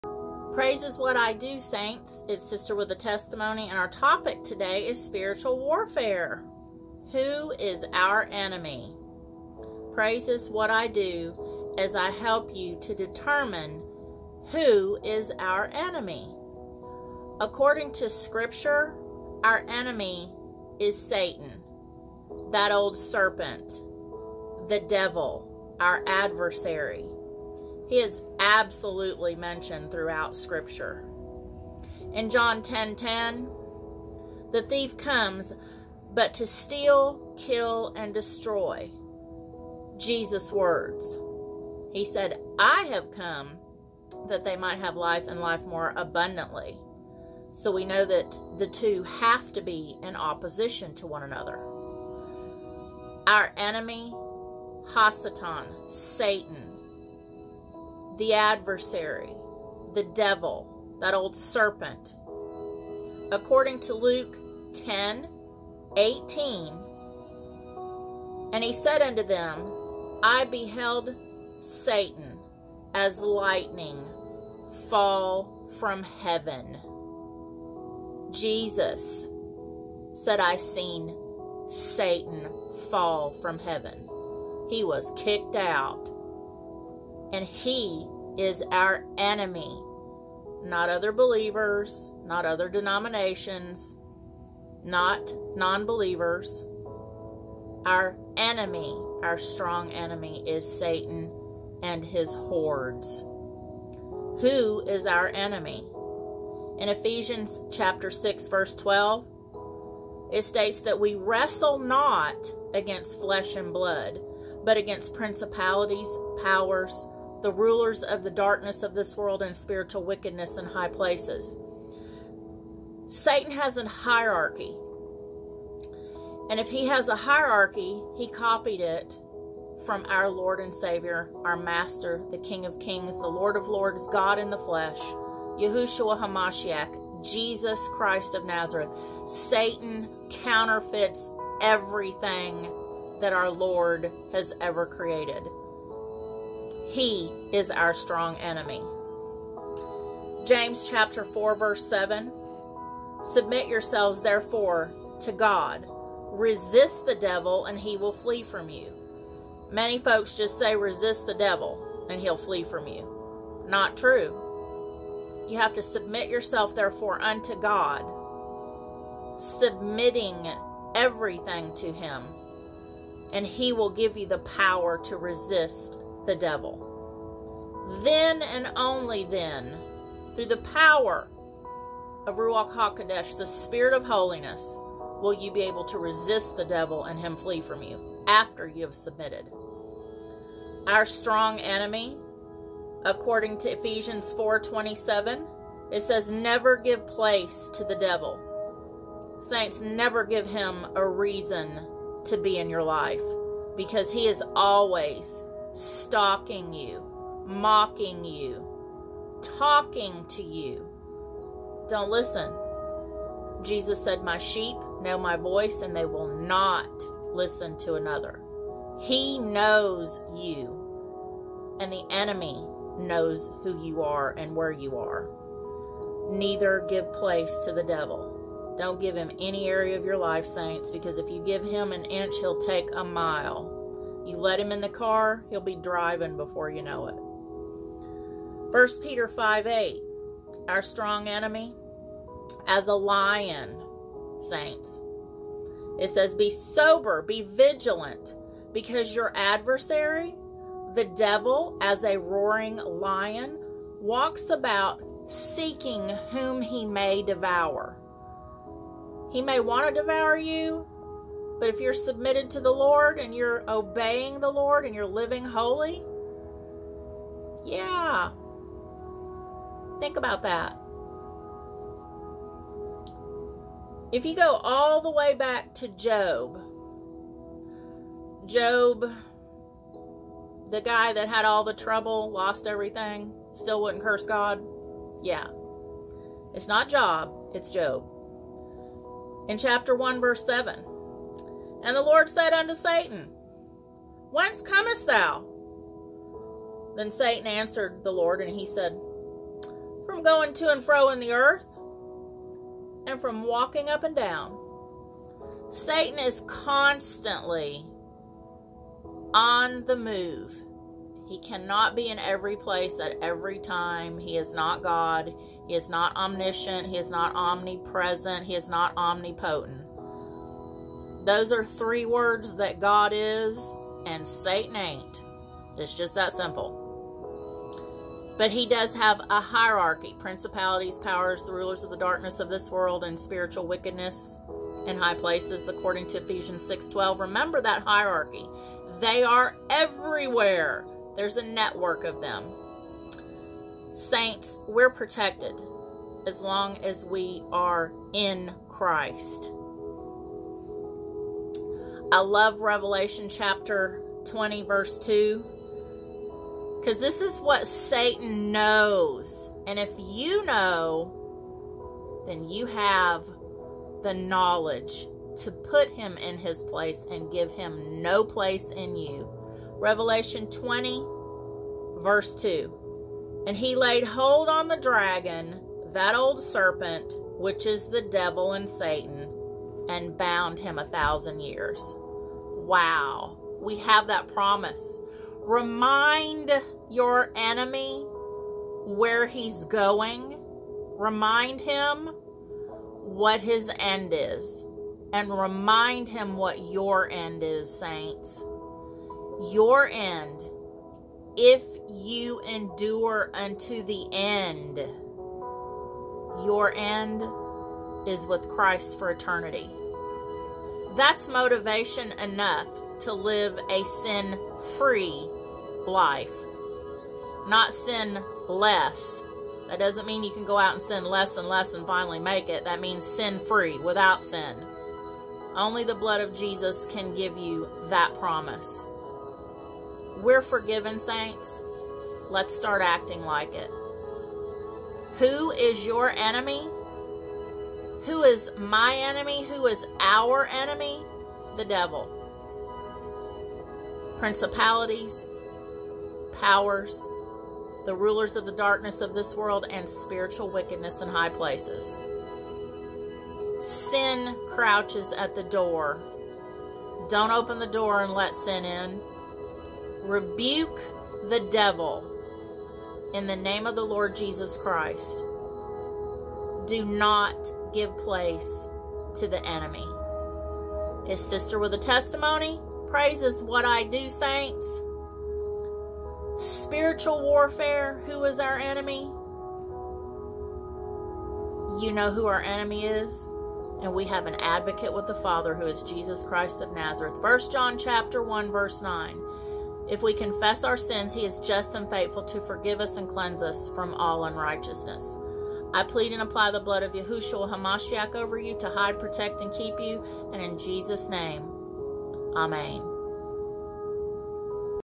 Praise is What I Do - SPIRITUAL WARFARE: WHO IS OUR ENEMY?
Praise is what I do Saints, it's Sister with a testimony, and our topic for today is: Spiritual Warfare -  Who is our enemy? Praise is what I do as I help you to determine who is our enemy. According to Scripture our enemy is satan. That old serpent, the devil, our adversary.